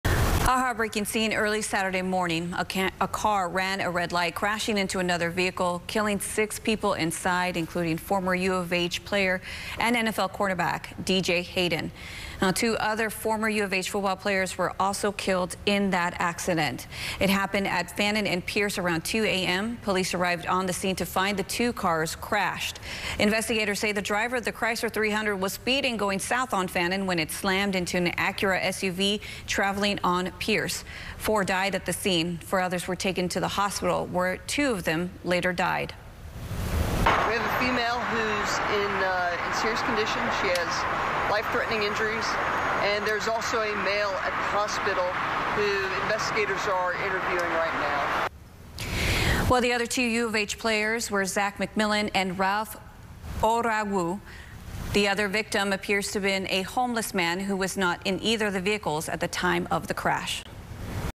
3 former UH players, woman die in car crash
Police say six people including former NFL player D.J. Hayden have died in a two-vehicle crash in downtown Houston. -- Idolina Peralez